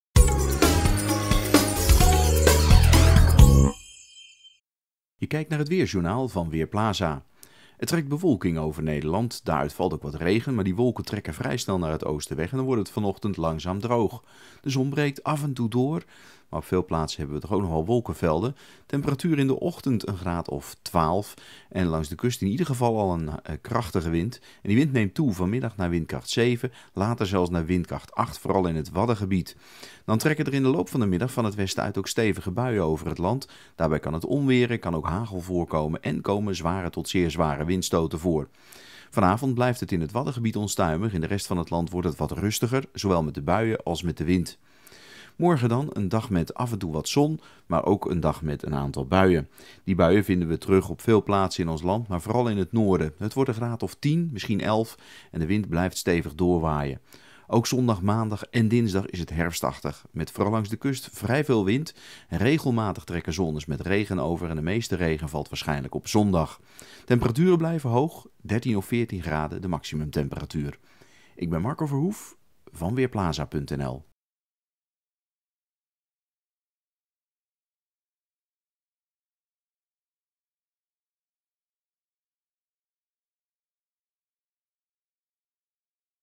Weerbericht vrijdagochtend: Herfstweer
In de ochtend trekt een gebiedje met buien van west naar oost over het land. Daarachter klaart het op en is er ruimte voor de zon. In de middag volgt vanuit het westen een nieuwe lijn met buien die snel oostwaarts wegtrekt. Dit kan vooral in de noordelijke helft van het land gepaard gaan met hagel, onweer en zware windstoten tot 90 km/u. Direct aan de kust zijn windstoten tot 100 km/u mogelijk. Er staat veel wind, 4 tot 8 Bft met in de kustprovincies kans op zware windstoten tot 90 km/h. Het wordt vanmiddag 12 tot 14 graden.